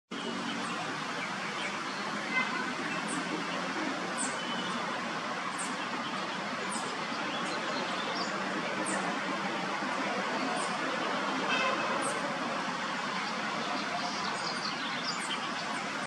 Amazilis amazilia
Colibrí visita cada flor para alimentarse del preciado néctar.